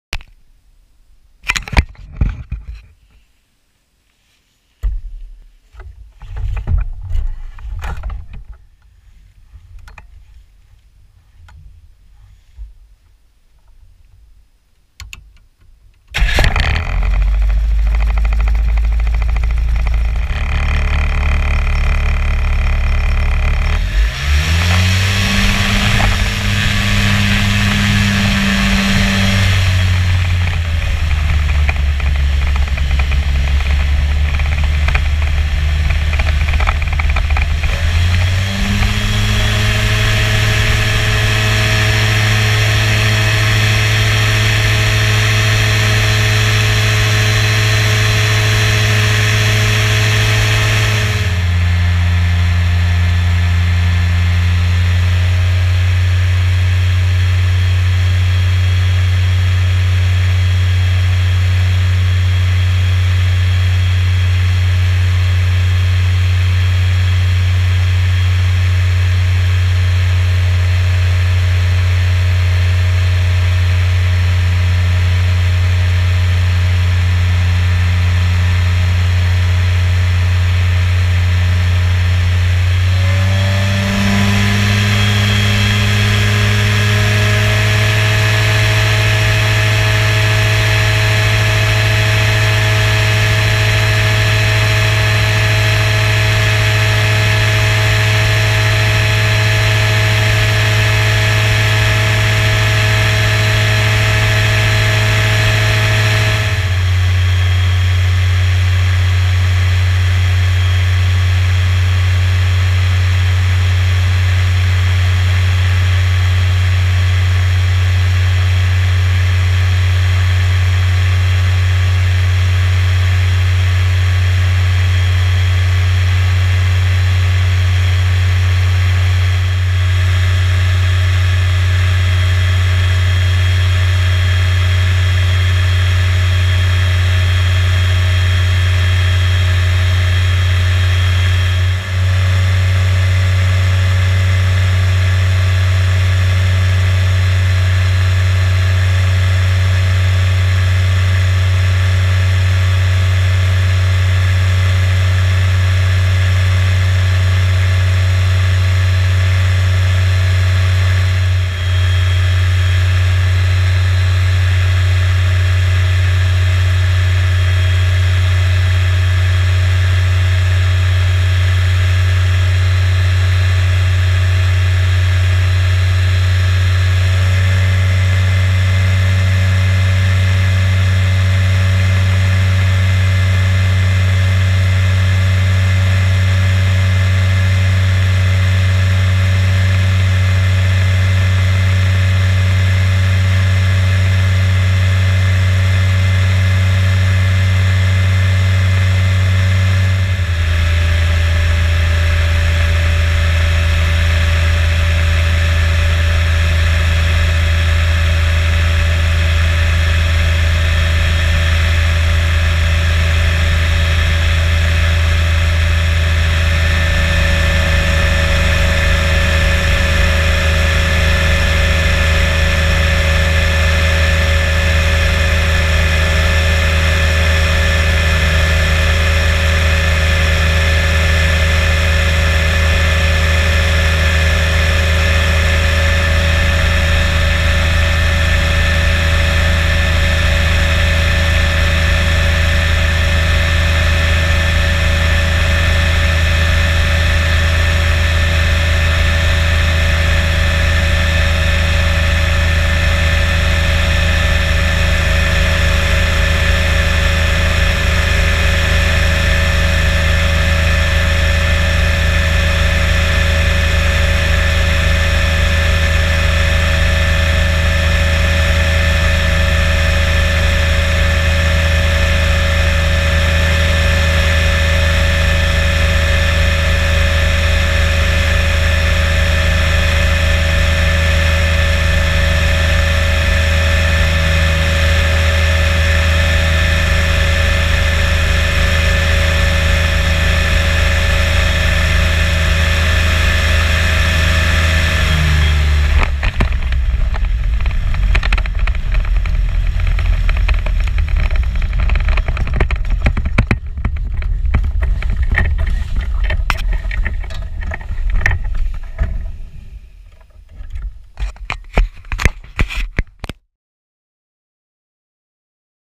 Tuesday Morning Flight Before Work
Taking advantage of the calm air on the Tuesday after Labor Day 2018.  Beautiful morning.